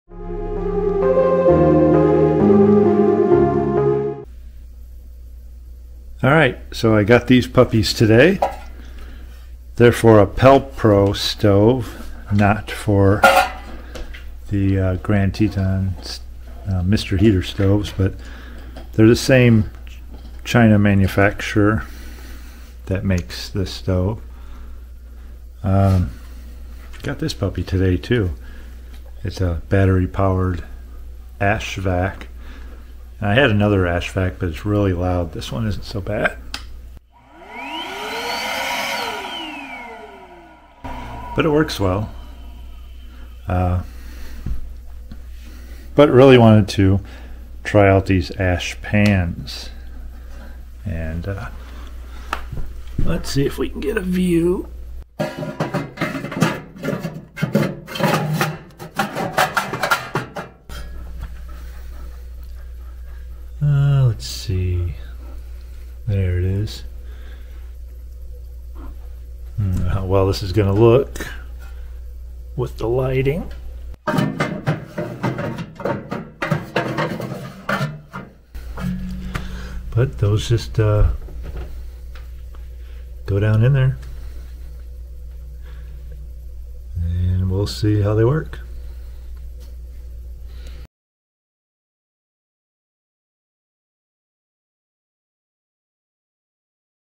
Grand Teton Teewinot pellet stove ash pans!
Short video about some ash pans that fit the Grend Teton pellet stove! These ash pans are made for PelPro stoves, but fit some of the Grand Teton stoves too, since the core stove is built by the same overseas factory.   They fit great, I'll letcha' know how they work out!

Battery Powered Ash Vac - Pellethead @EarthSense Ash Vault Pro
Ash Pans - Criditpid store on Amazon - See screenshot in video